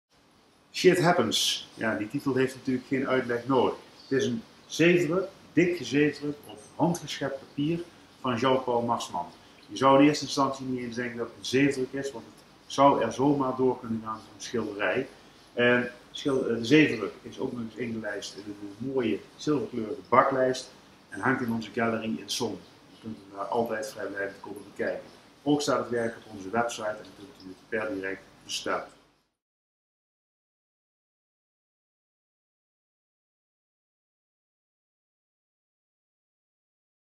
Jean Paul Marsman | Shit Happens
Leuke kleurrijke ingelijste zeefdruk van Jean-Paul Marsman. Verkrijgbaar bij Jules Verne Art